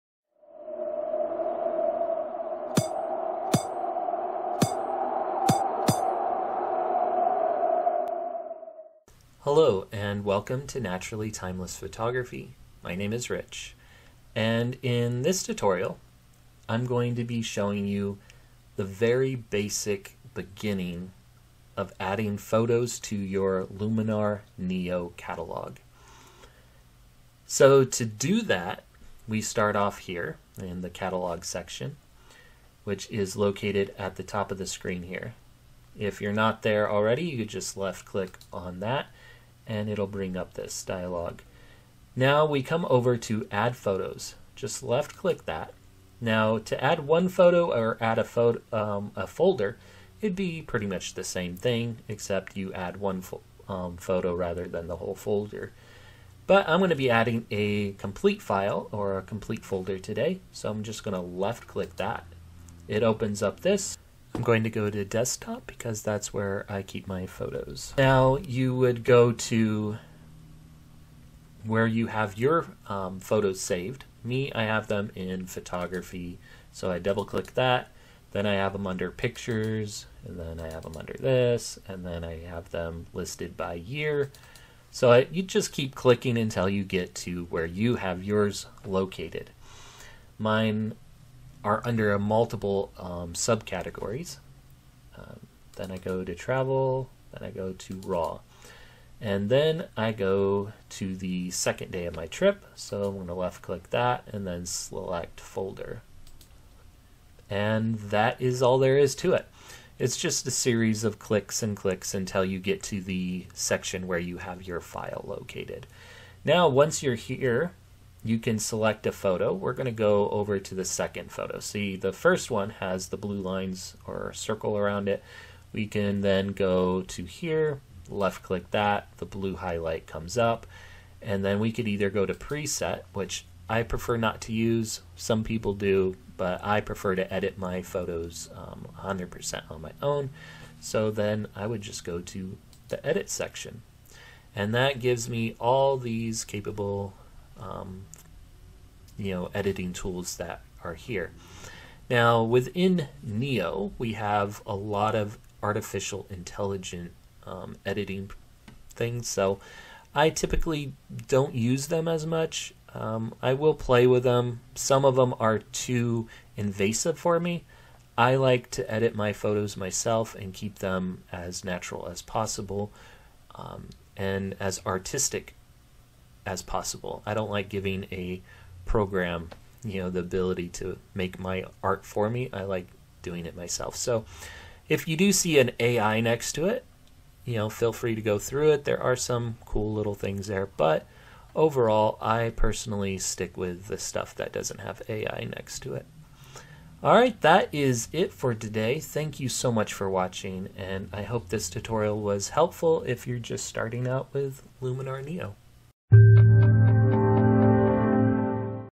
1. Luminar NEO - Adding Photos To Your Catalog
This is a basic tutorial on how to add photos to your Luminar NEO catalog.  Definitely for beginners just starting out with a program or editing.